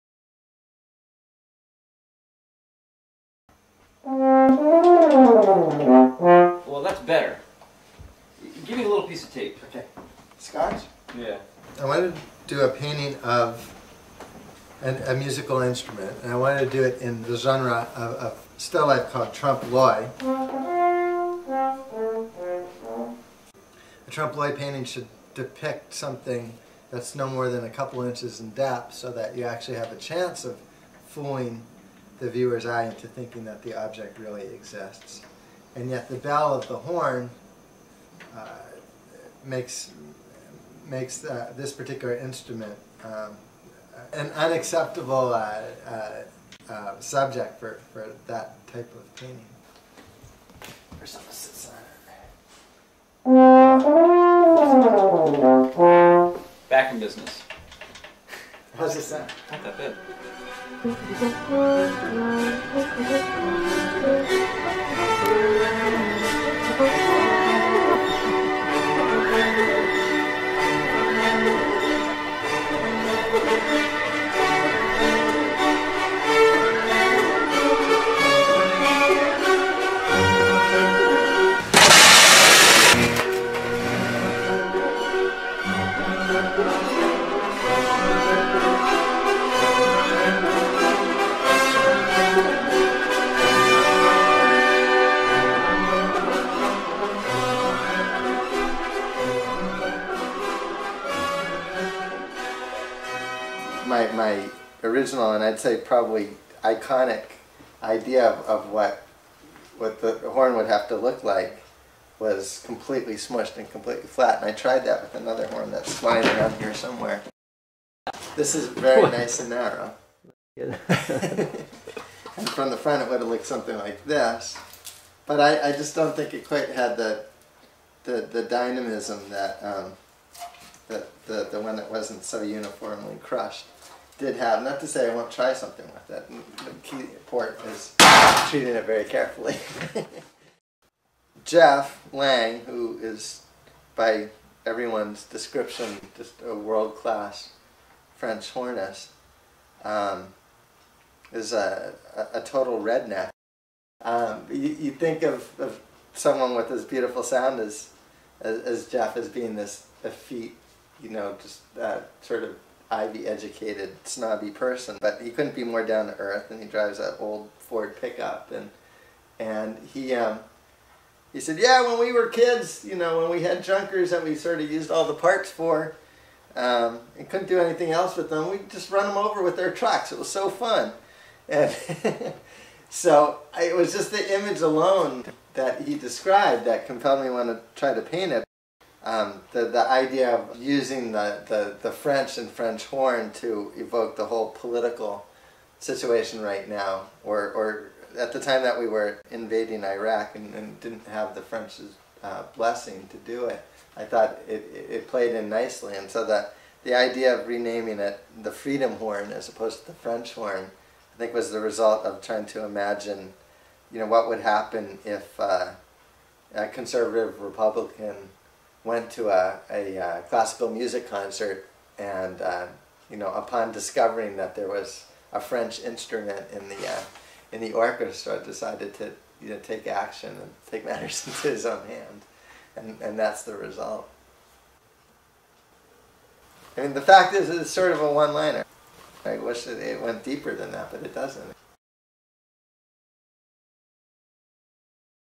Freedom Horn
Creation of the painting "Freedom Horn" by Mikel Glass with the assistance of Philadelphia Orchestra hornist Jeffrey Lang.